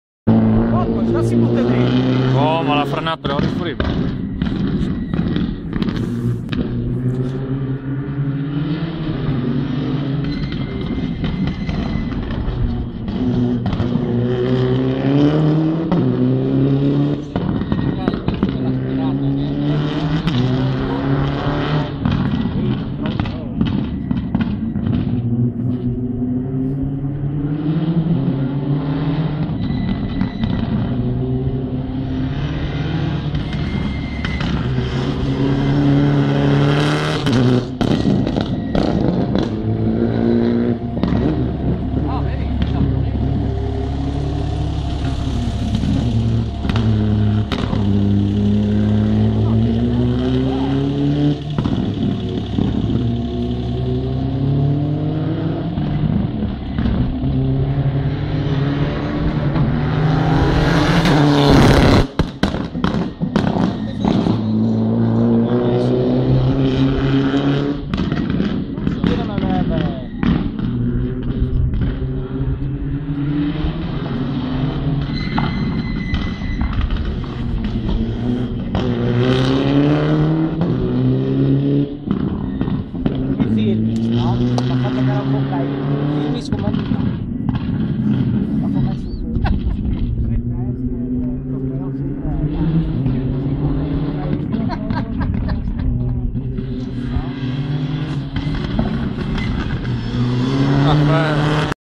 Test Marco_ Citroen DS3 R3T FriulMotor [HD]
Test De Cecco Marco sulla nuova Citroen DS3 R3T FriulMotor in pista!